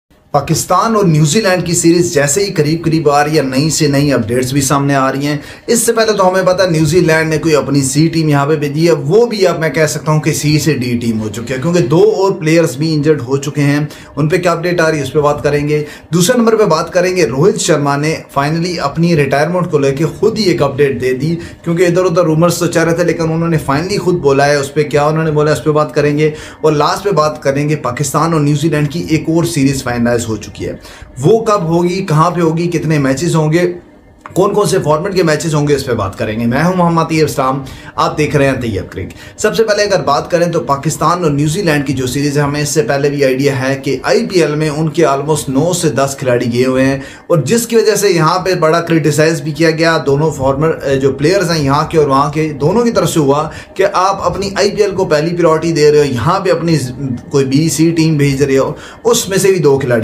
2 big changes in Pak vs Nz series
2 big players ruled out of pak vs nz | Rohit sharma on retirement| Pakistan vs Nz series schedule

tayyabcric
pakistan cricket news today
rohit sharma ipl
Nz players of of series
Pak new series